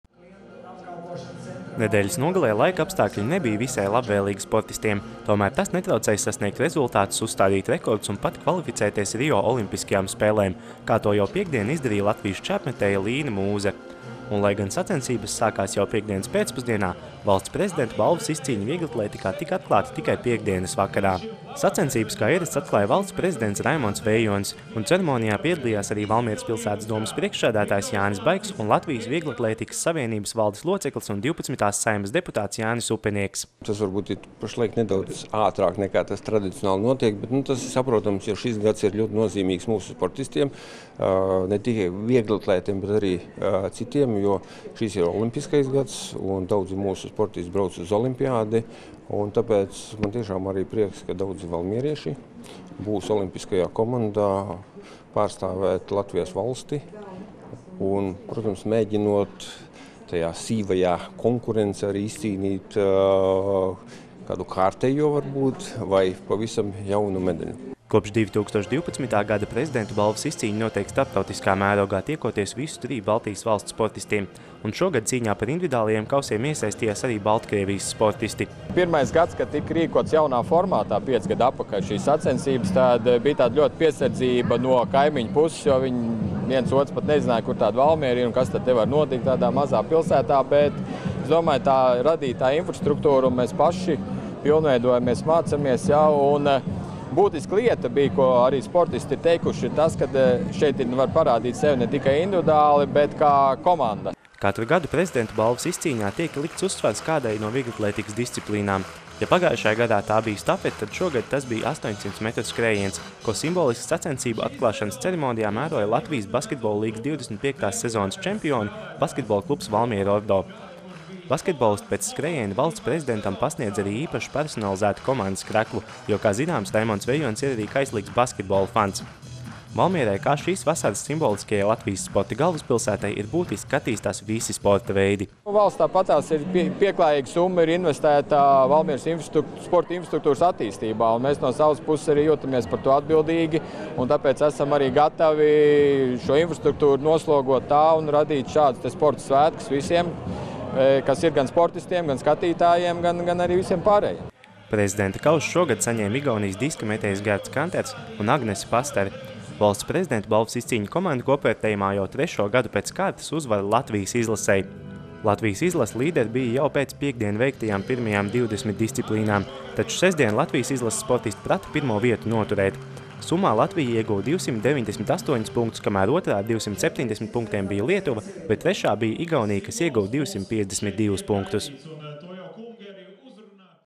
Valsts prezidenta balvas izcīņa vieglatlētikā
10. un 11.jūnijā Jāņa Daliņa stadionā notika vienas no tradīcijām bagātākajām vieglatlētikas sacensībām Baltijā – Valsts prezidenta balvas izcīņa. Šogad sacensības norisinās jau 22.reizi, savukārt piekto gadu komandu vērtējumā tajās spēkiem mērojas Baltijas valstu vieglatlētikas izlases. Individuālo dalībnieku sastāvu šogad papildina vairāk nekā 30 sportistu no Baltkrievijas.